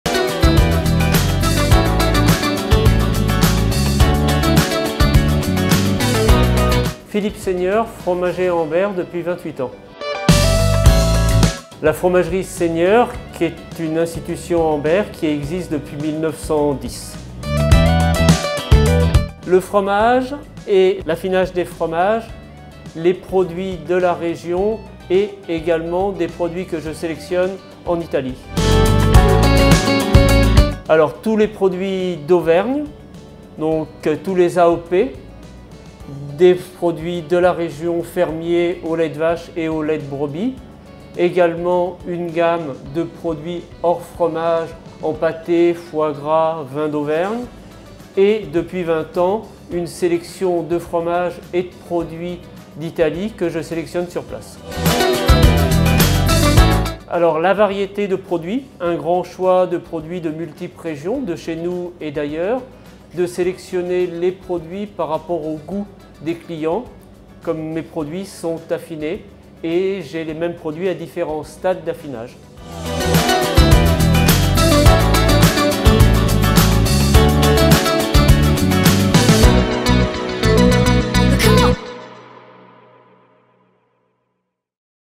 La Fromagerie SEIGNEUR à Ambert est membre des Vitrines d'Ambert Livradois Forez !
Découvrez les commerçants et artisans adhérents de l'association LES VITRINES D'AMBERT LIVRADOIS FOREZ en vidéo !

Aujourd'hui, nous découvrons la fromagerie SEIGNEUR, 4, place de la Pompe à AMBERT ! Visite avec Philippe SEIGNEUR...

Ouvert :
Mardi au Samedi  8h30 - 12h30 / 14h30 - 18h45
Jeudi 8h30 - 12h30 / 15h00 - 18h45
Dimanche 8h45 - 12h00